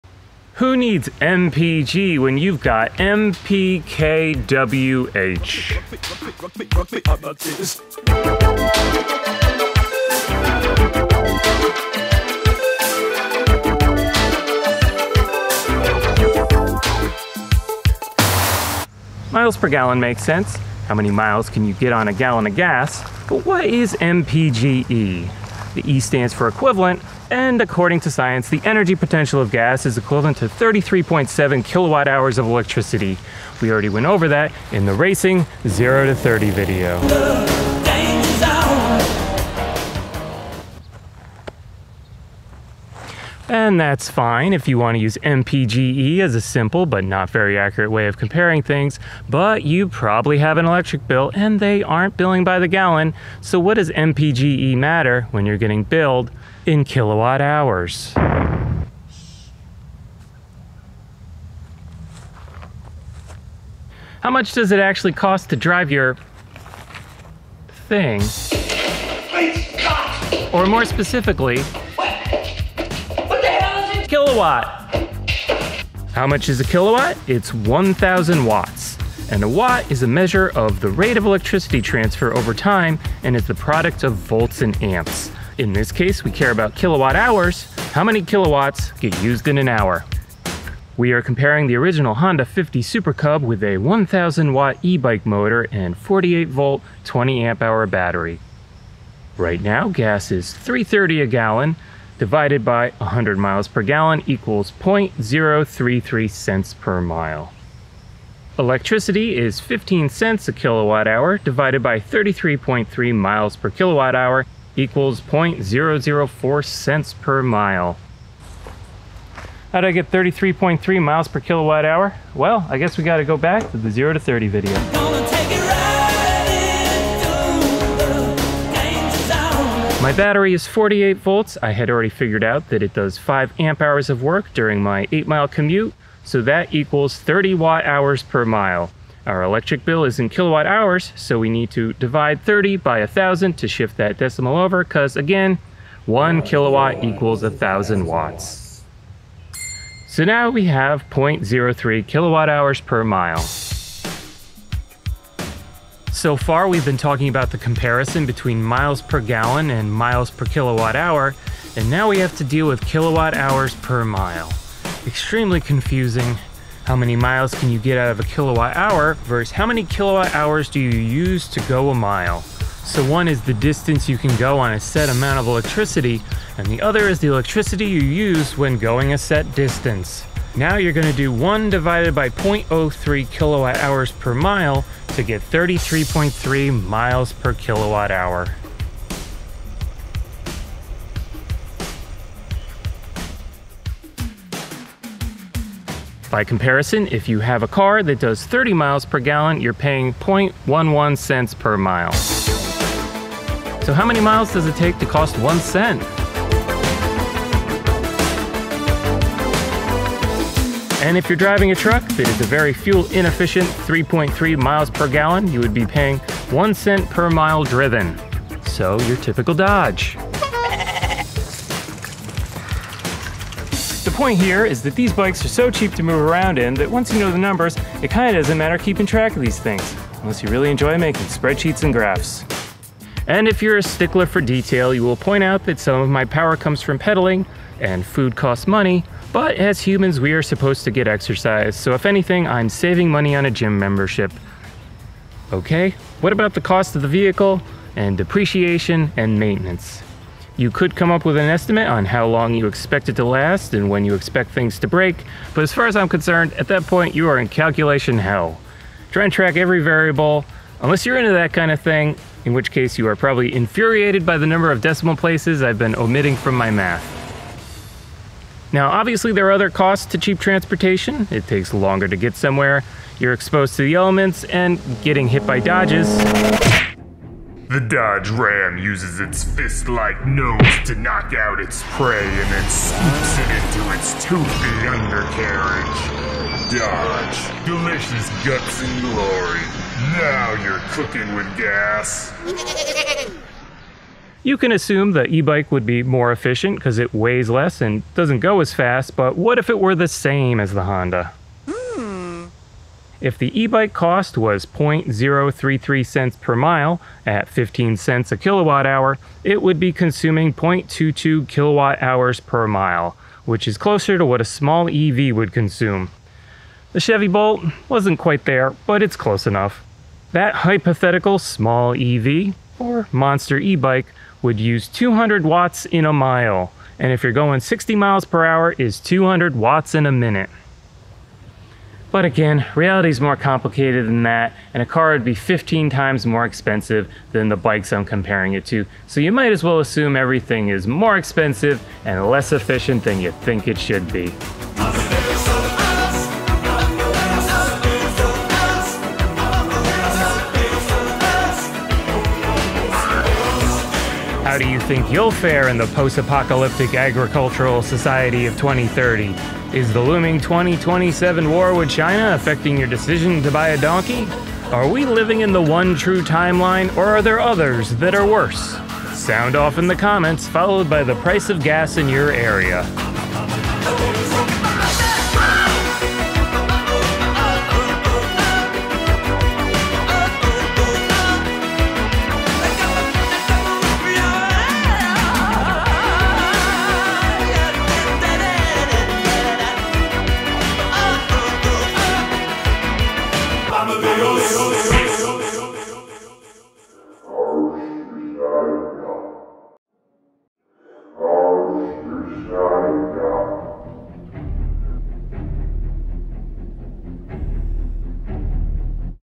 MPG vs MPkWh vs MPGe Cost of Gas vs Cost of Electricity
Gas mileage vs Electricity efficiency. How much is the cost of Gas compared to cost of Electricity with eBikes, Motorcycles, Cars and Trucks. This video references topics covered in the video links bellow.

** TIME CODES **
00:00 Intro
00:19 MPGe
01:03 What is a Kilowatt
01:30 Honda 50 Super Cub MPG vs eBike kWh
02:35 Kwh per Mile vs Miles per kWh
03:15 How Many Miles Can You Travel on a Penny
03:42 It’s so Cheap that Tracking Doesn’t Matter
04:54 A Small EV Car is the almost same efficiency as a Scooter
05:44 End

ebike, mpg, mpge